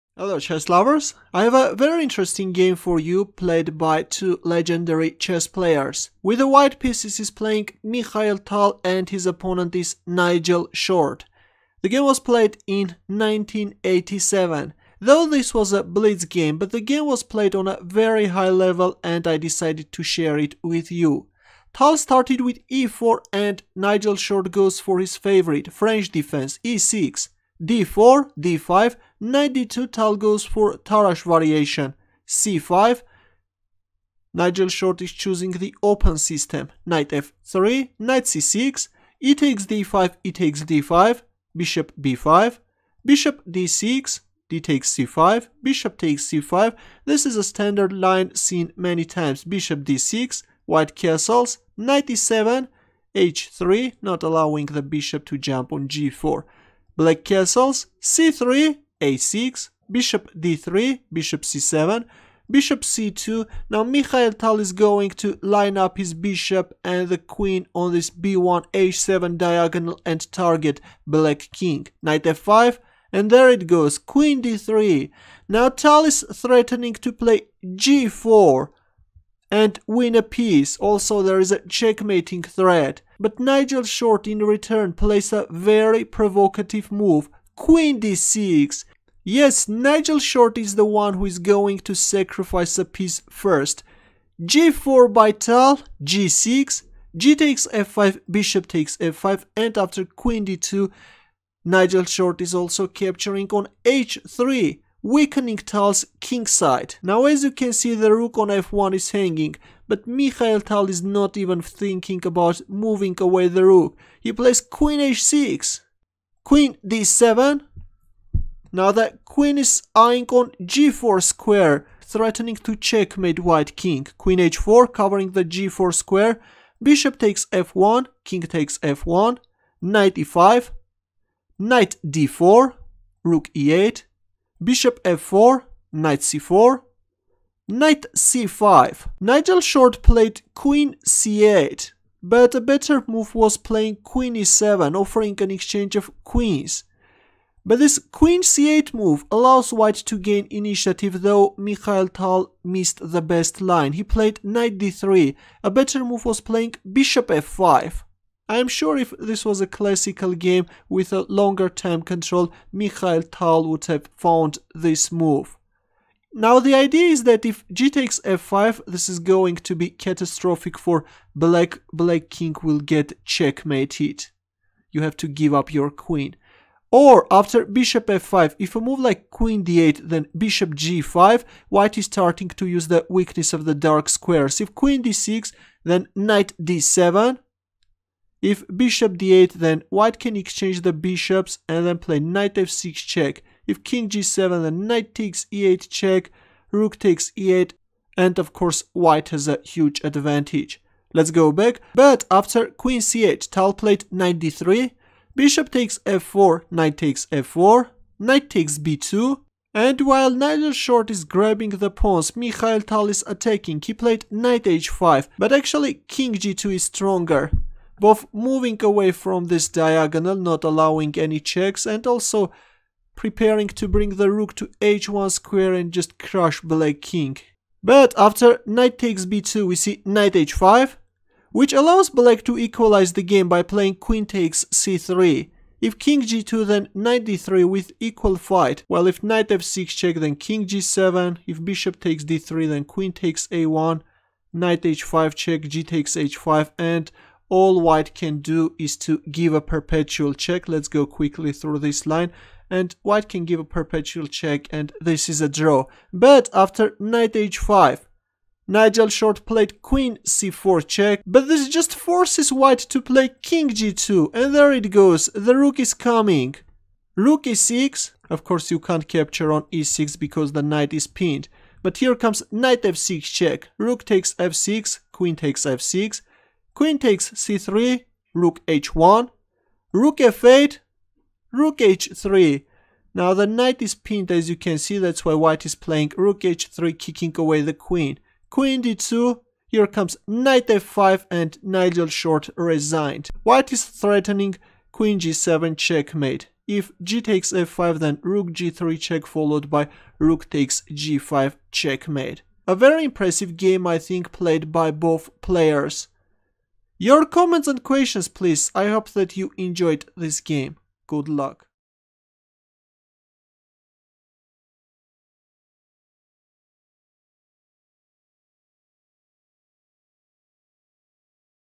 Clash Of Titans! Mikhail Tal vs Nigel Short
Mikhail Tal (2605) vs Nigel Short (2615)
SWIFT - World Championship blitz, 1987 Brussels BEL, Apr-26
French Defense: Tarrasch Variation. Open System Main Line (C09)

1. e4 e6 2. d4 d5 3. Nd2 c5 4. Ngf3 Nc6 5. exd5 exd5 6. Bb5 Bd6 7. dxc5 Bxc5 8.Nb3 Bd6 9. O-O Nge7 10. h3 O-O 11. c3 a6 12. Bd3 Bc7 13. Bc2 Nf5 14. Qd3 Qd6 15. g4 g6 16. gxf5 Bxf5 17. Qd2 Bxh3 18. Qh6 Qd7 19. Qh4 Bxf1 20. Kxf1 Ne5 21.Nfd4 Rfe8 22. Bf4 Nc4 23. Nc5 Qc8 24. Nd3 Bxf4 25. Nxf4 Nxb2 26. Nh5 Qc4+ 27.Kg2 Re6 28. Nf6+ Rxf6 29. Qxf6 Qxc3 30. Rh1 Rf8 31. Rh3 Qd2 32. Nf5
1-0